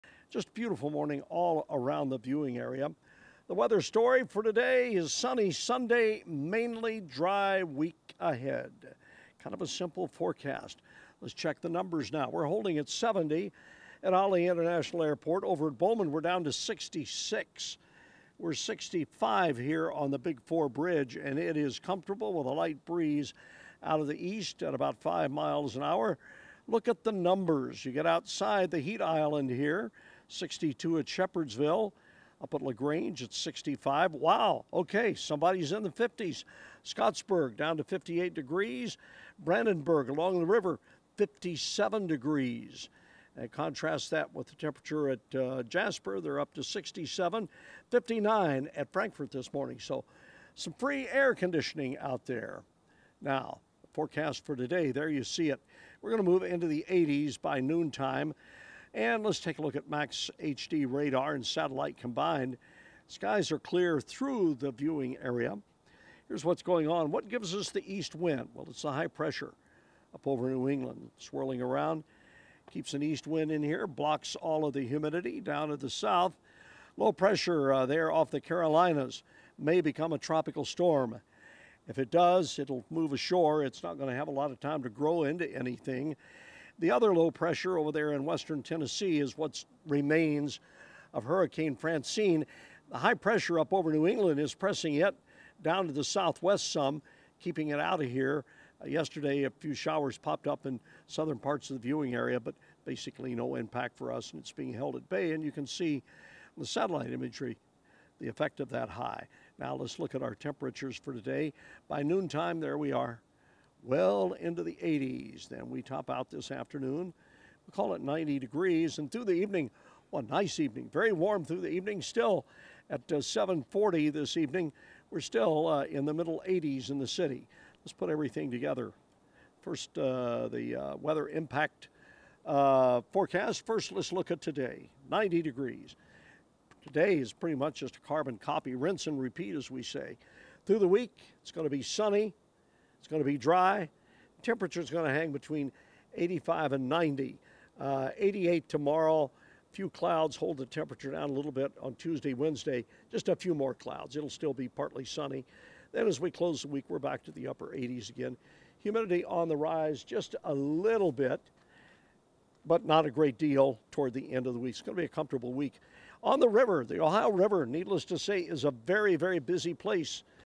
Plenty Of Sunshine And Warm | WHAS11 7:00 a.m. weather Sept. 15, 2024
Sunny days, not too humid with highs in the upper 80s... no rain in sight